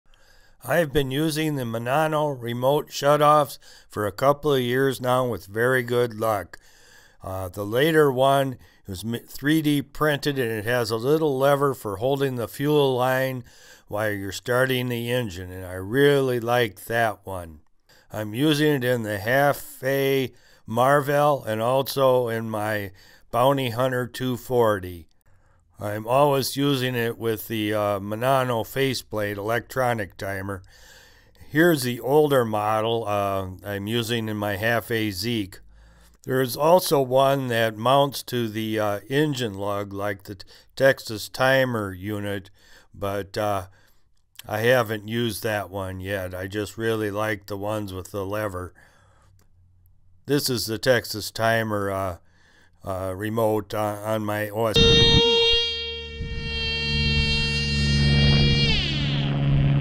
Menanno Remote Shutoff
Showing the Menanno remote engine shutoffs I have been using.